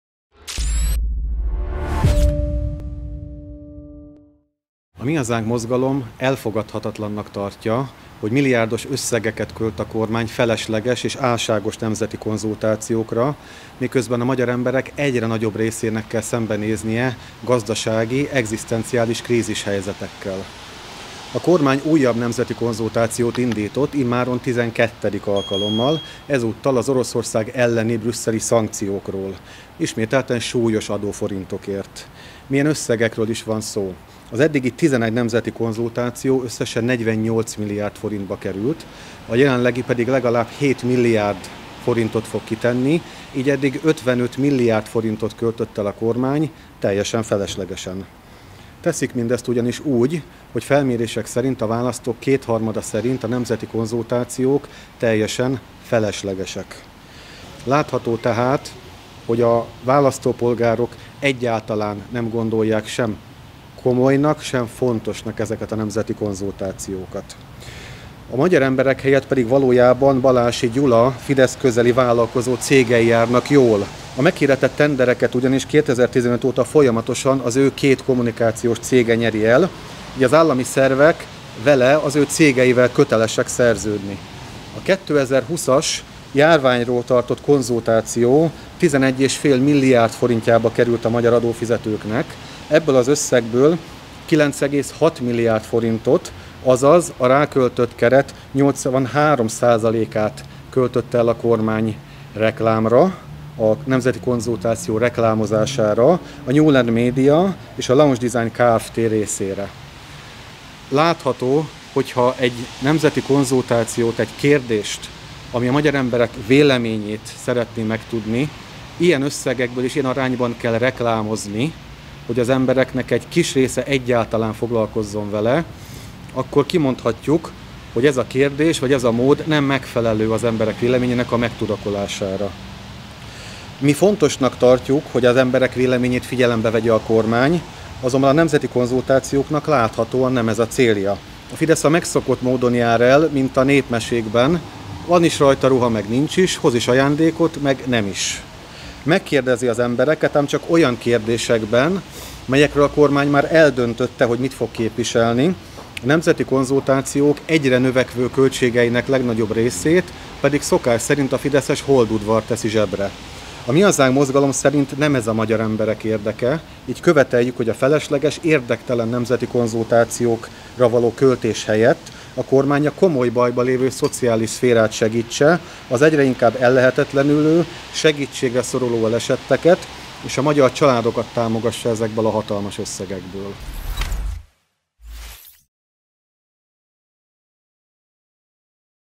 Mennyit költhetünk nemzeti konzultációra?
A Mi Hazánk Mozgalom szerint a nemzeti konzultációk jelen formájukban feleslegesek. A Kormány olyan kérdéseket tesz fel, melyekről már eldöntötte, mit fog képviselni és melyek nem igazán érdeklik a magyar emberek többségét, míg a konzultációk lebonyolítása közben adómilliárdok kerülnek a fideszes vállalkozók zsebébe. Követeljük, hogy újabb pénzkidobások helyett a kormány a bajban lévő szociális szférára  és  a magyar családok támogatására költse ezeket a hatalmas összegeket.